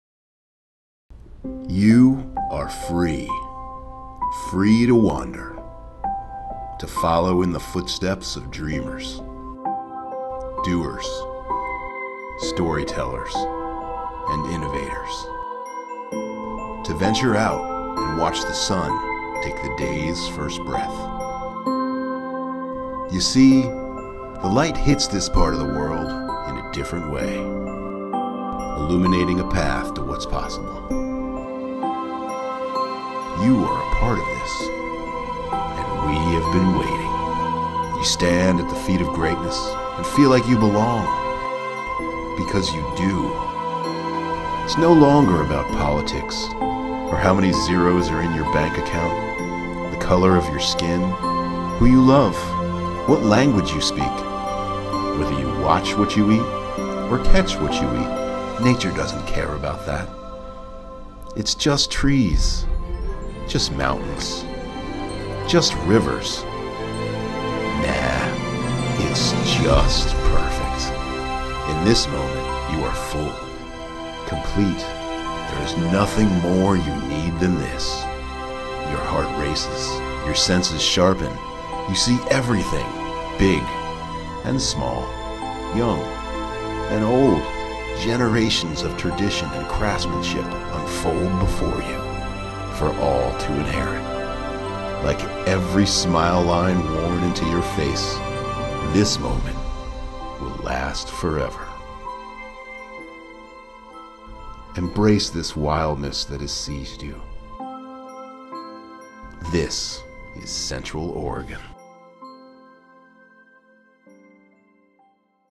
"This is Central Oregon"
In Central Oregon, you're free to wander, to follow in the footsteps of dreamers, doers, storytellers, innovators. To venture out and watch the sun take the day’s first breath. You see, the light hits this part of the world in a different way. This is Central Oregon...